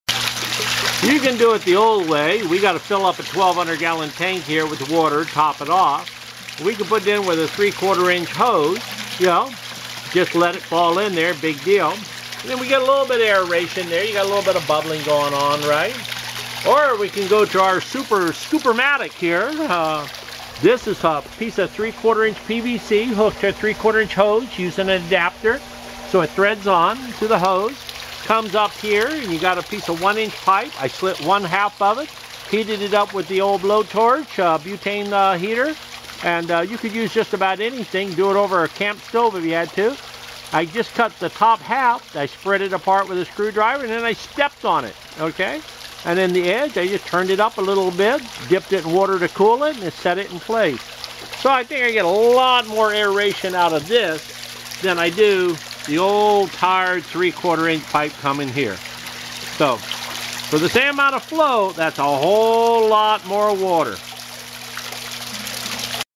Glenn has made a new invention to make more aeration for the water going into the fish tank.
Glenn Martinez of Olomana Gardens has created a way to make more aeration from a pvc pipe that he slit, flatten, and curved that makes the water shoots upward and makes more bubbles. Pretty inventive on his part.